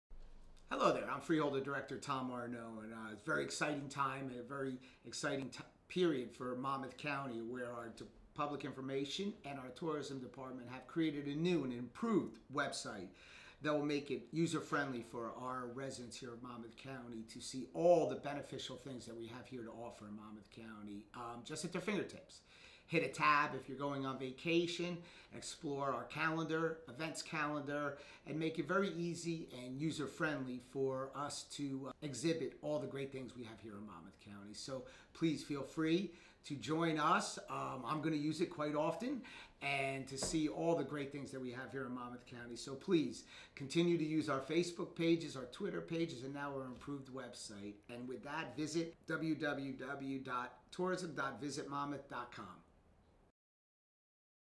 Freeholder Director Thomas A. Arnone Introduces New Monmouth County Tourism Website!
The Monmouth County Board of Chosen Freeholders is marking the first day of summer by announcing the launch of the new Monmouth County Tourism